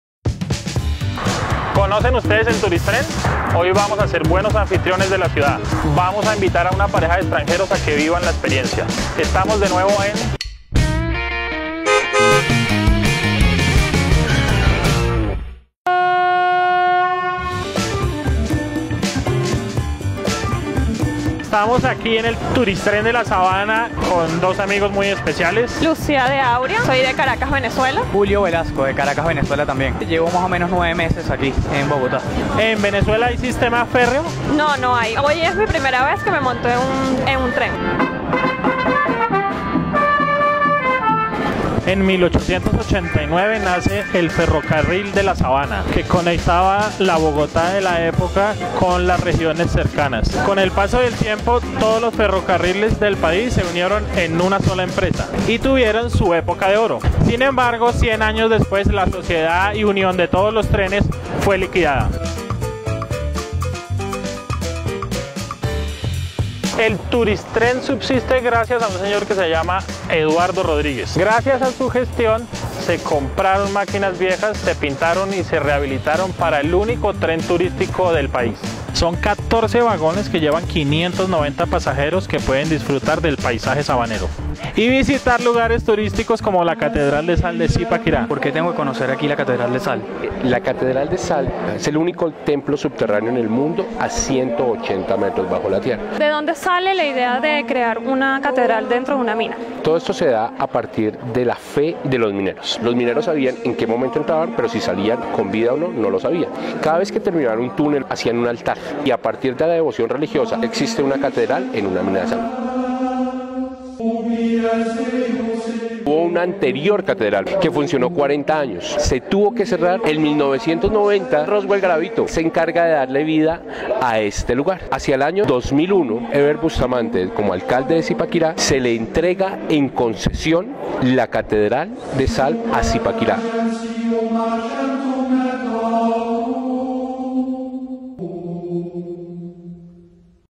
Visita a la Catedral de Sal de Zipaquirá en el Turistren de la Sabana
Conozcan la primera maravilla de Colombia a nivel turístico ubicada a una hora de Bogotá. Llevamos a una pareja de venezolanos para que vivieran la experiencia.